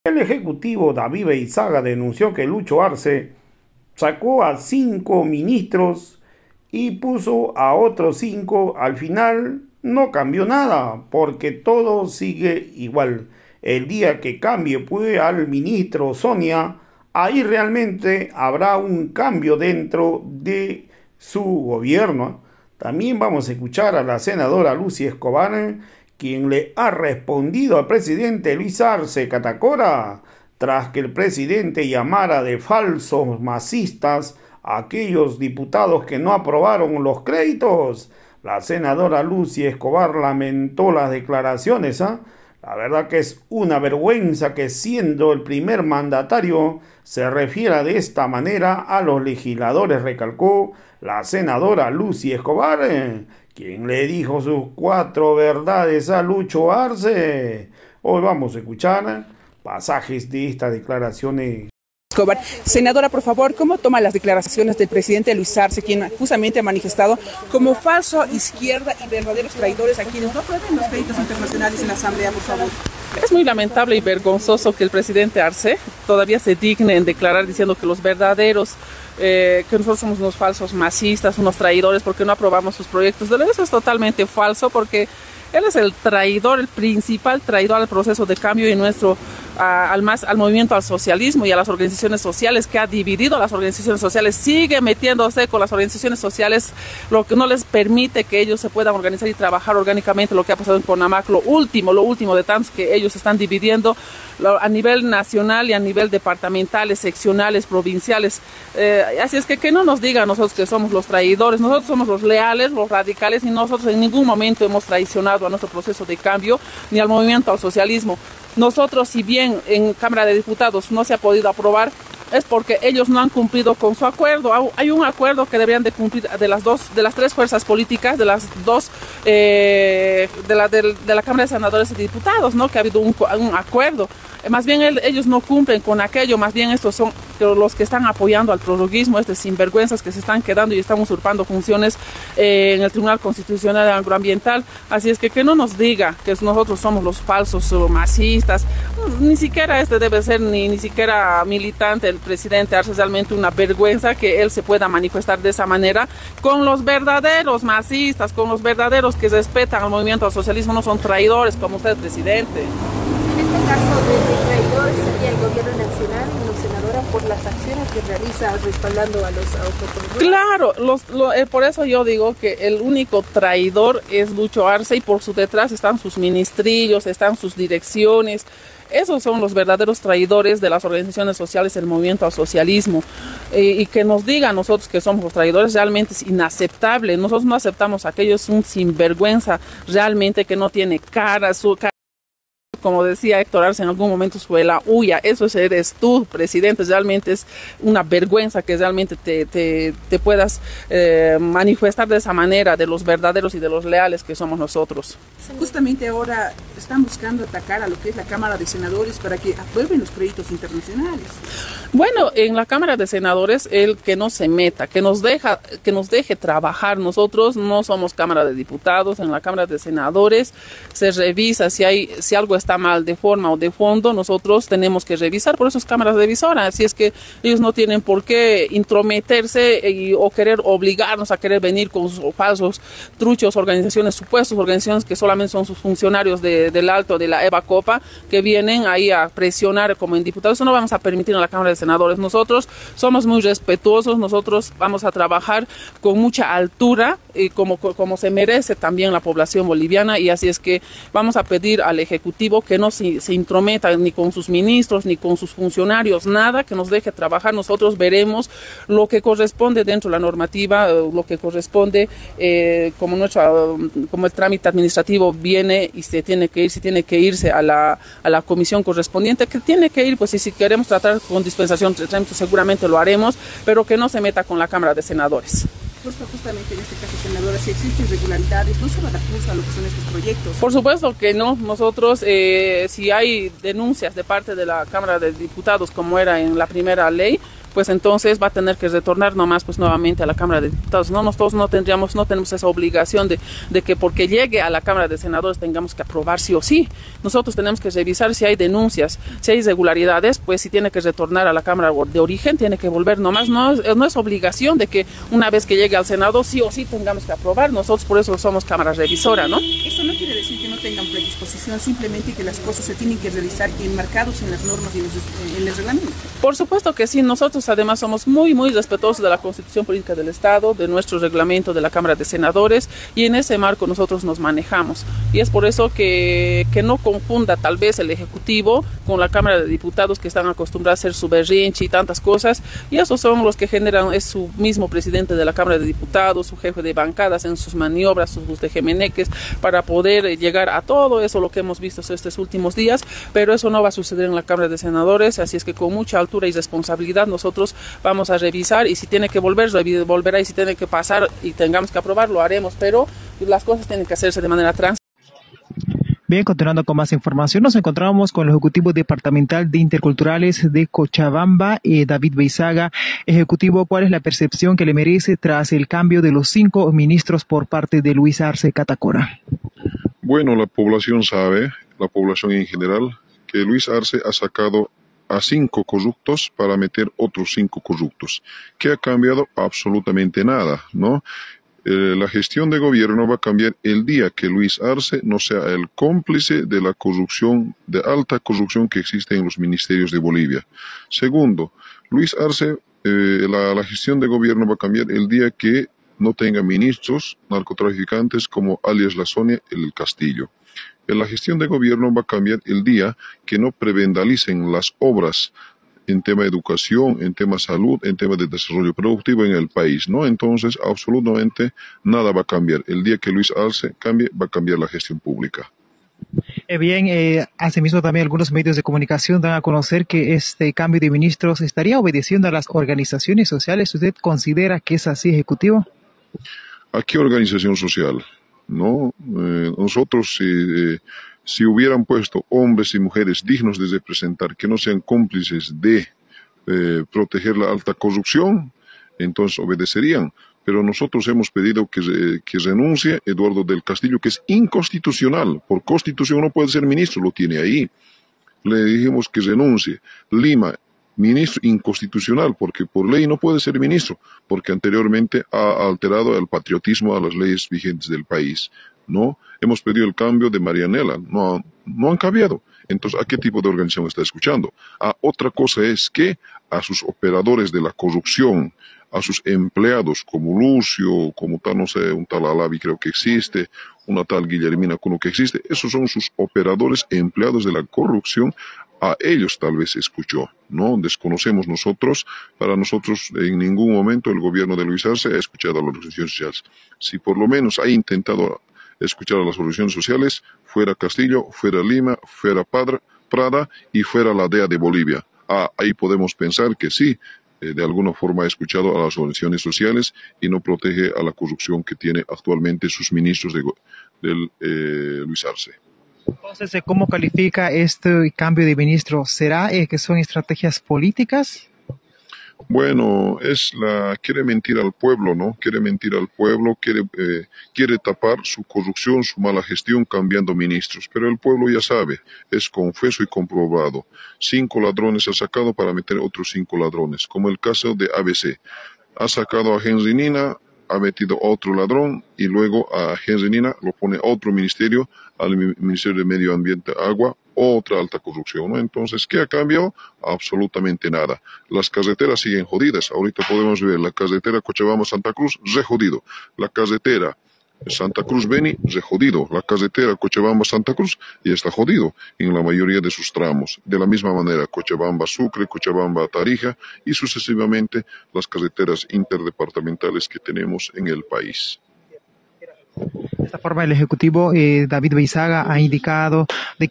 SENADORA ESCOBAR CUESTIONA PRESIDENTE ARCE AL LLAMAR FALSO MASISTA POR NO APROBAR CREDITOS..
EJCUTIVO DAVID VEIZAGA TODO SIGUE IGUAL CON LOS MINISTRO PARA EXISTA CAMBIO TIENE QUE SACAR MINISTROS CUESTIONADOS..